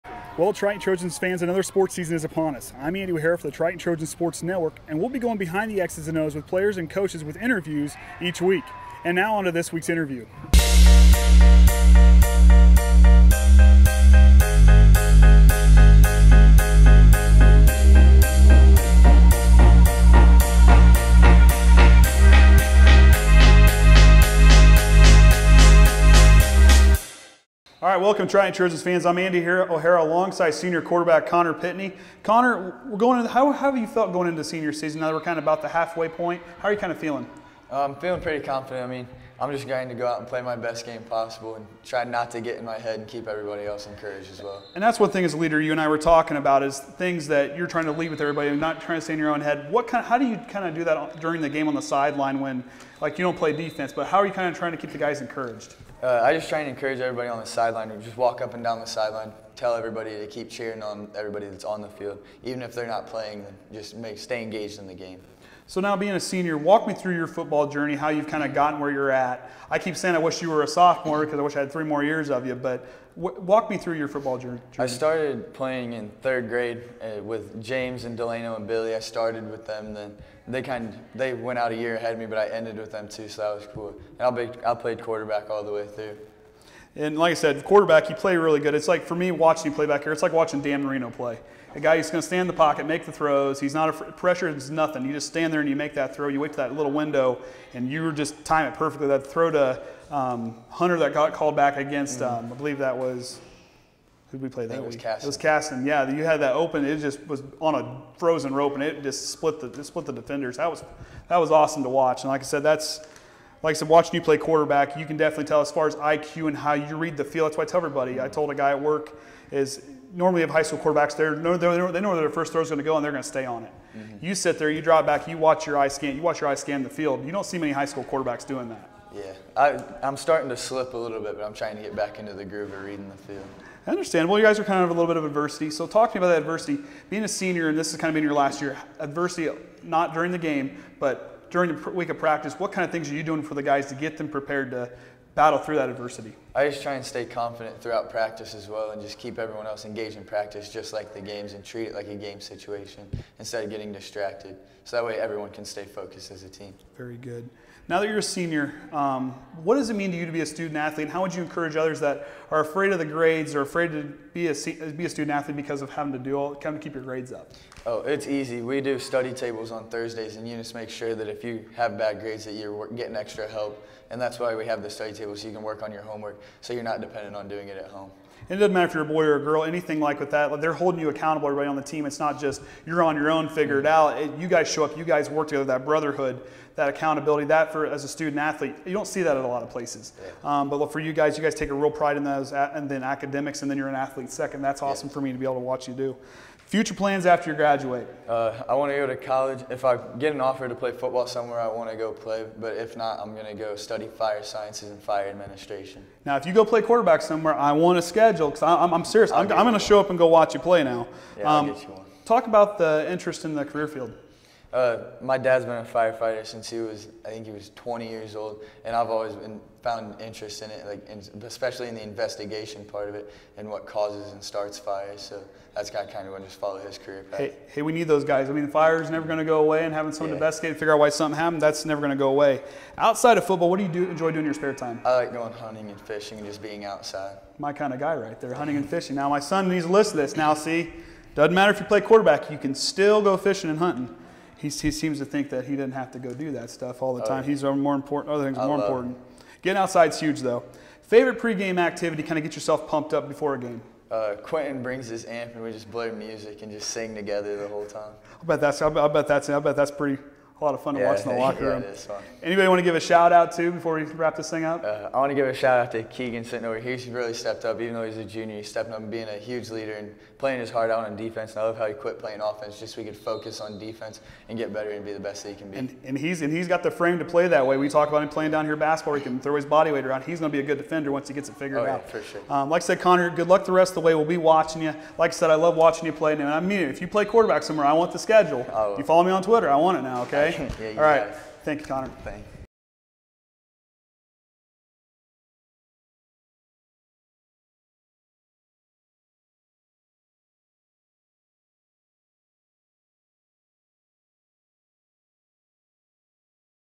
Connor Pitney 🏈 2019 Triton Football Spotlight
TTSN's Andy O'Hara speaks with senior quarterback Connor Pitney and how he began his journey playing football.

TTSN unpaid Volunteers
🎤 The Voice of Triton Athletics: Andy O'Hara
🎬 Broadcast Producer: Orion Lemler
💯 Stats, Commentary & Technical Support: Kenny Barnhart
🎥 Cameras: Travis Barnhart, Margo Lemler, Orion Lemler
📺 Advertising: Mason McIntyre